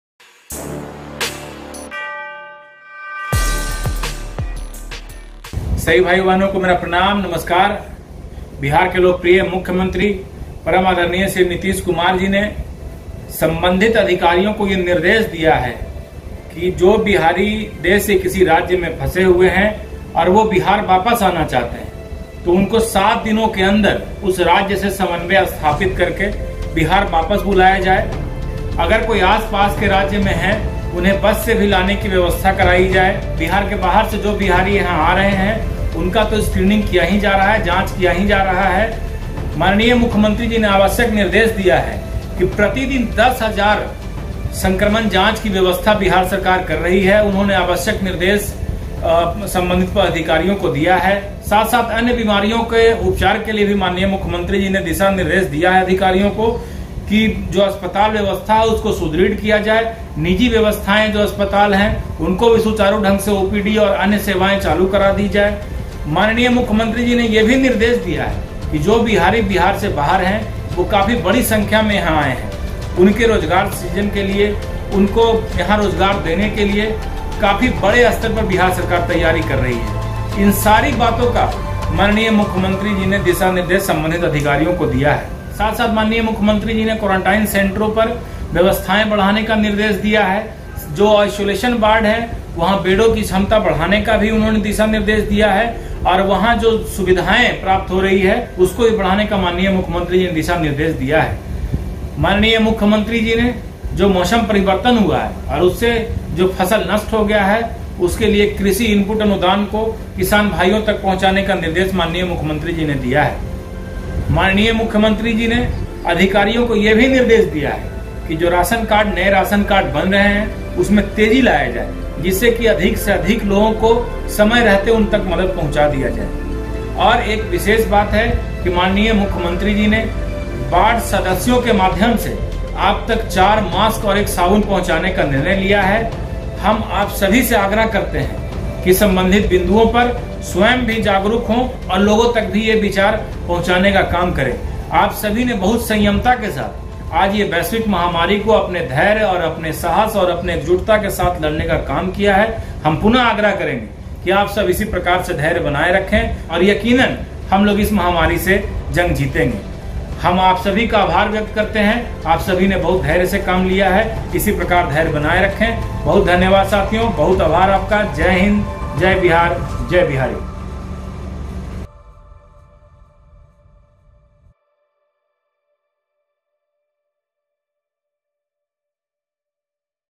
Sri Nitish Kumar Ji CM Bihar Gave Necessary Instruction to Concerned Officers - Chandan Kumar Yadav
माननीय मुख्यमंत्री आदरणीय श्री नीतीश कुमार जी ने अधिकारियों को बिहार और बिहारियों के सुरक्षा और सुबिधा हेतु आवश्यक निर्देश देते हुये कहा की 07 दिनों के अंदर इच्छुक व्यक्ति को बिहार लाया जया और कई जरूरी आदेश माननीय ने दिया - चन्दन कुमार यादव प्रदेश प्रवक्ता जनता दल यूनाइटेड बिहार नगर निकाय